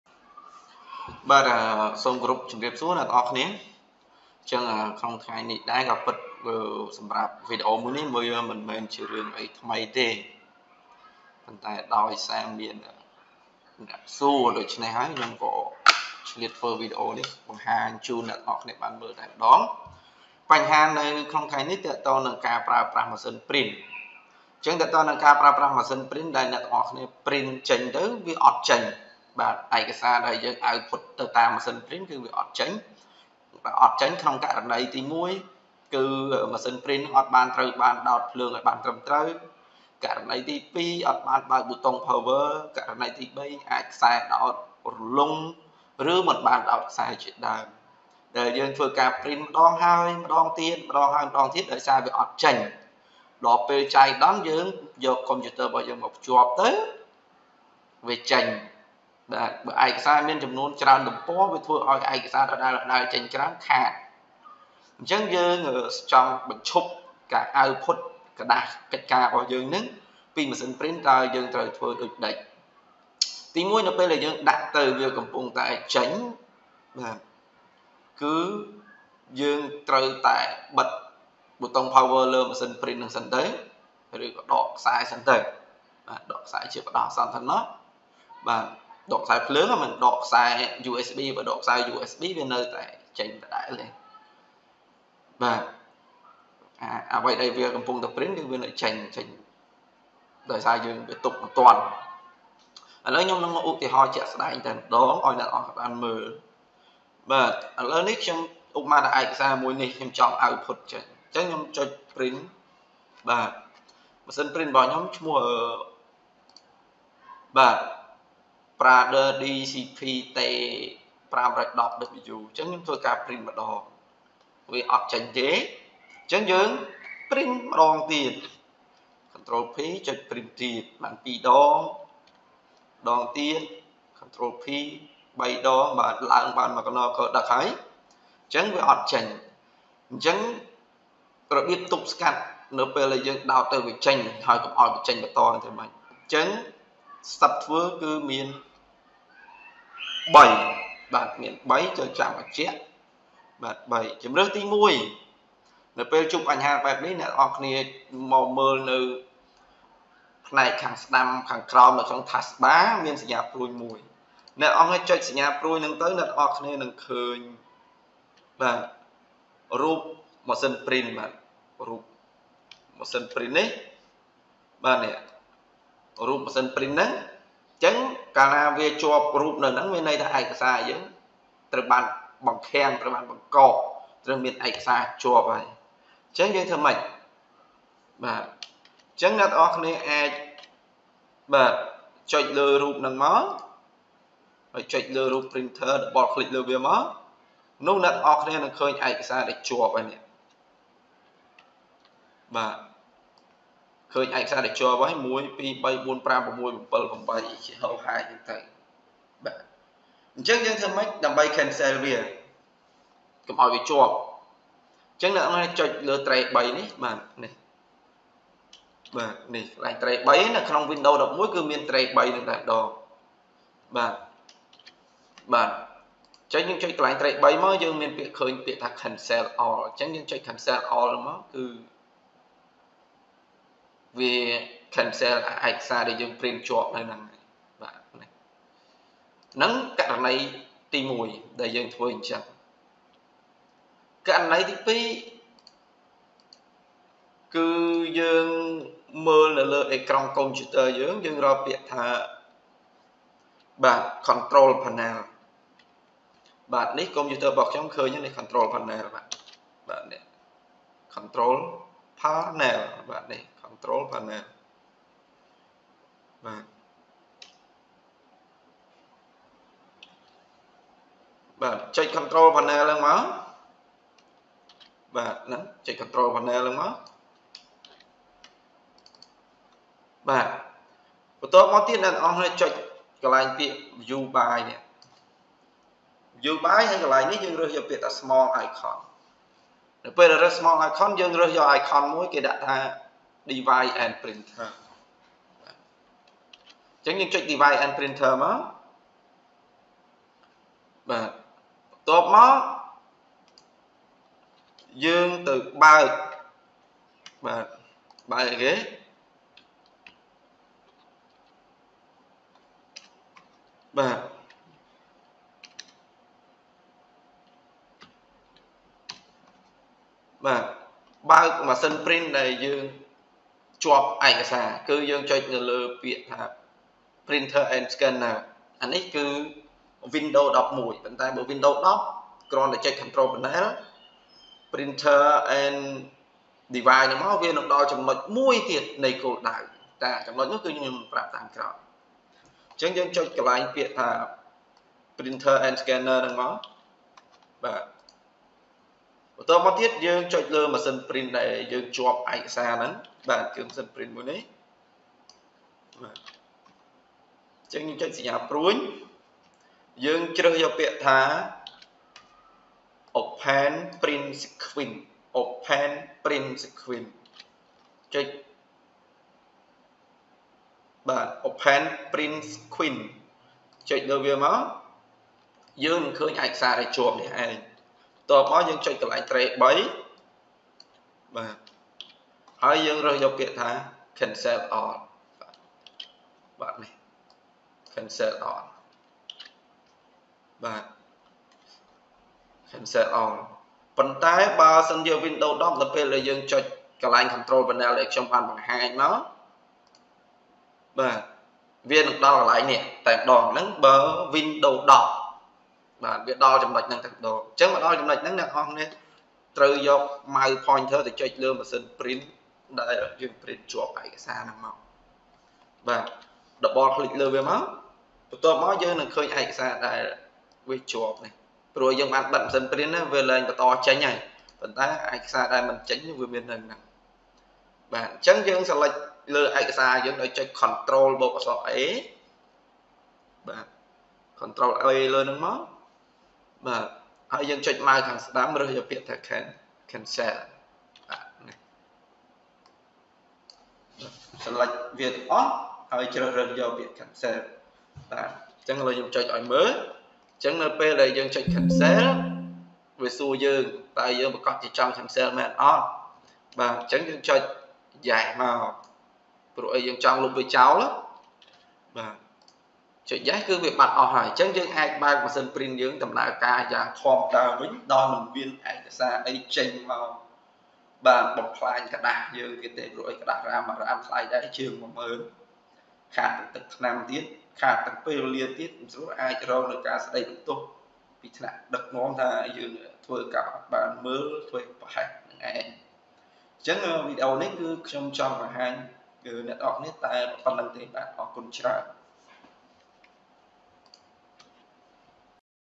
How to Cancel Documents from Printer device
របៀបធ្វើការលុបឯកសារចេញពីម៉ាស៊ីនព្រីន ដែលយើងបានធ្វើការព្រីនមិនចេញ